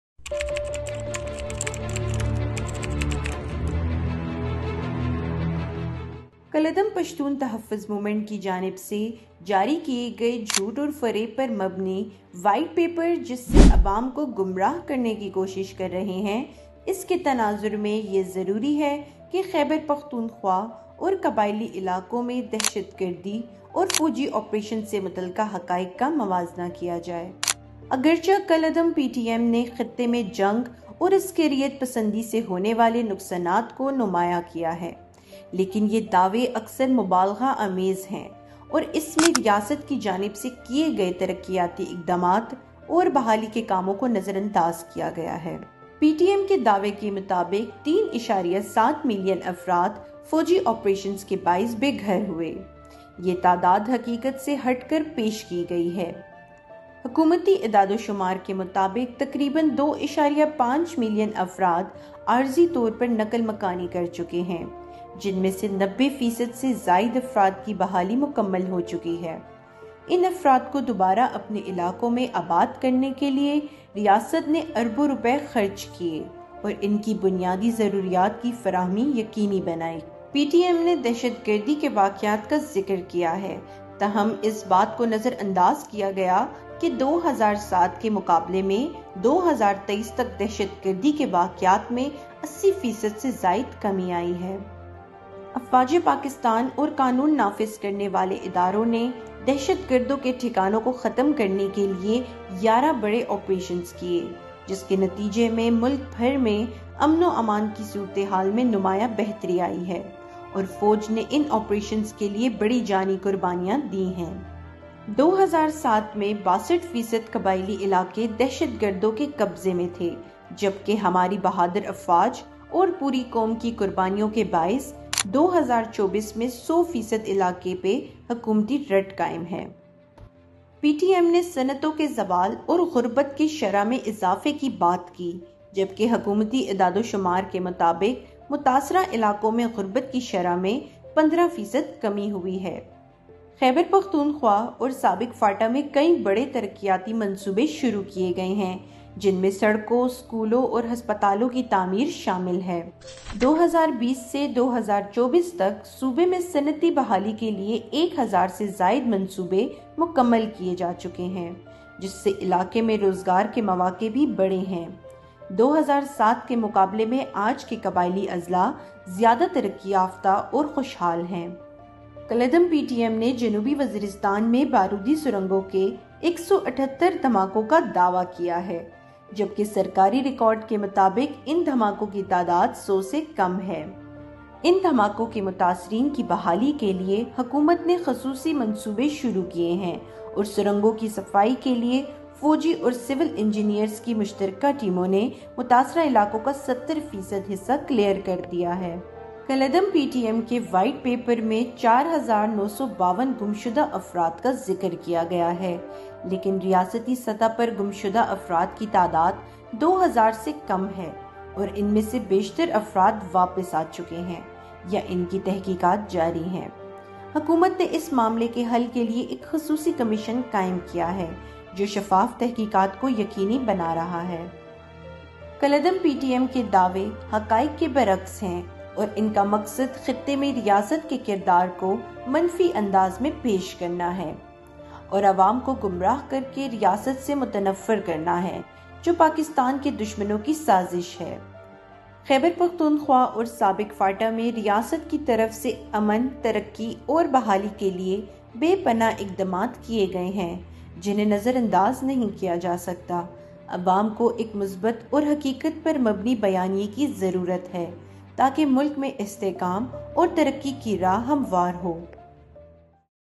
State's Development and Peace Restoration Efforts and Banned PTM's Propaganda.
In this video, we explore the development initiatives and efforts for restoring peace led by the government in various regions. We also discuss the misinformation and propaganda spread by the banned Pashtun Tahafuz Movement (PTM). Watch to gain a better understanding of the state's efforts and the ground realities of the situation.

Key Topics Covered:

Government's development projects
Steps taken to restore peace
The role of banned organizations in spreading propaganda
Debunking false narratives